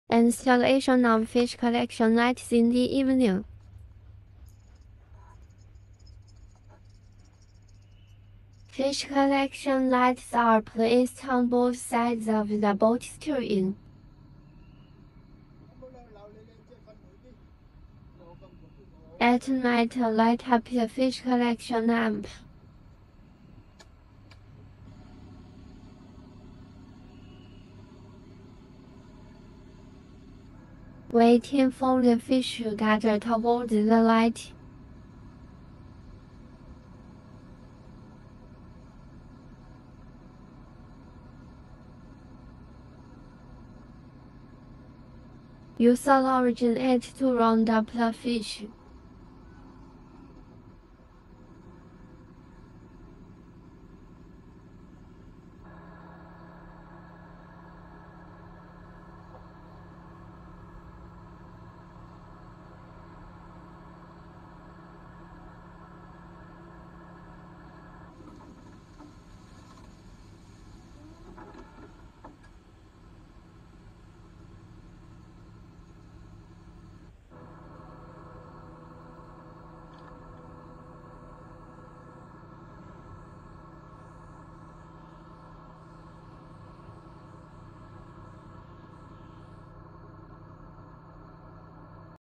Fishing at night
Smart people. Teach us how not to waste the efficacy of fishing light, and suggest that your fishing boats can be designed in this way.